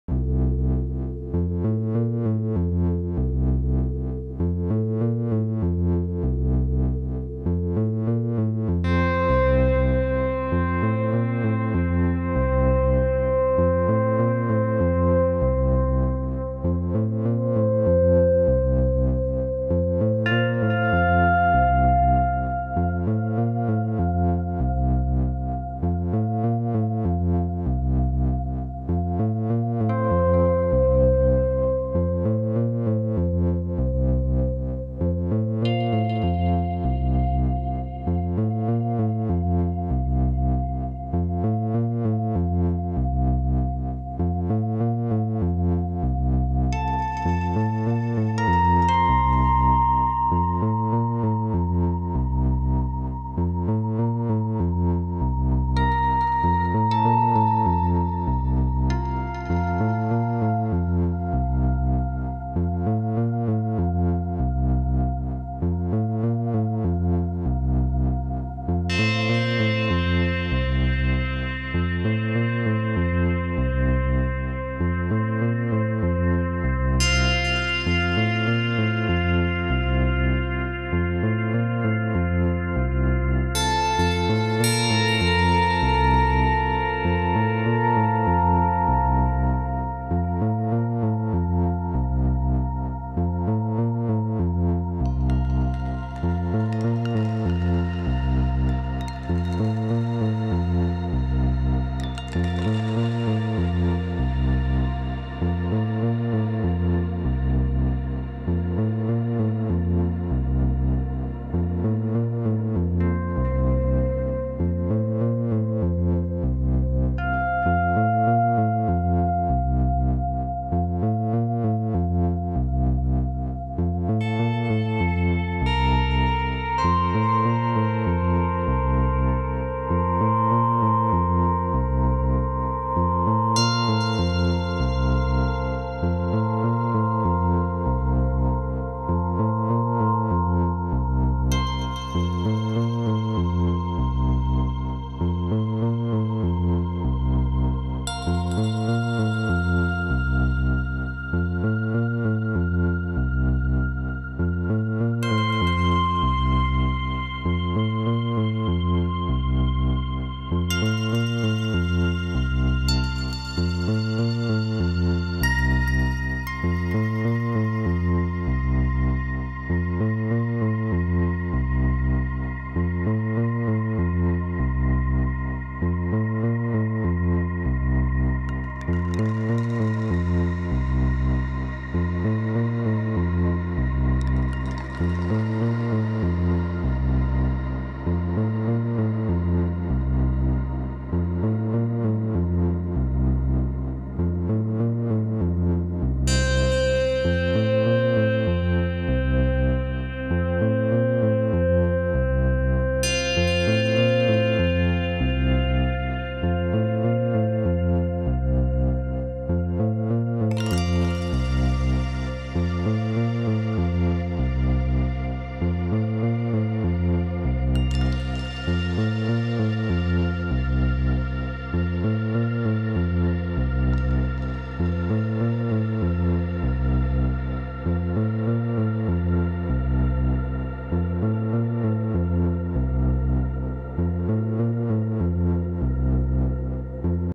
Eurorack Fridays
Simple patch this morning.
Bass line from the Arturia Microbrute.
I am manually sending notes to Mutable Instruments Rings from the Intellijel Quadratt which gets quantized by the Intellijel μScale.
Some manual wiggling of various Rings parameters.
Rings is set to four note polyphony in sympathetic strings mode.
Rings odd and even outputs go into the Empress Echosystem which is in Drunk Ewok (lost in space) mode.
Then through the Focusrite Scarlett interface and into the Zoom H5 recorder. The Zoom is in audio interface mode and sends the audio to the iPhone SE which is used to record the video.